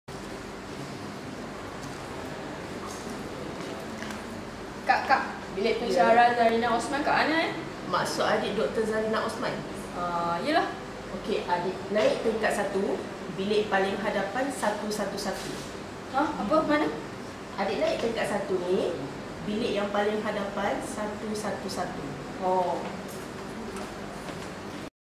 Kak, kak, bilik pensyarah Zarina kat mana?
Video untuk tugasan kursus ZZZT1052 Pembangunan Diri 1 komponen komunikasi, Pusat Pengajian Umum, Universiti Kebangsaan Malaysia.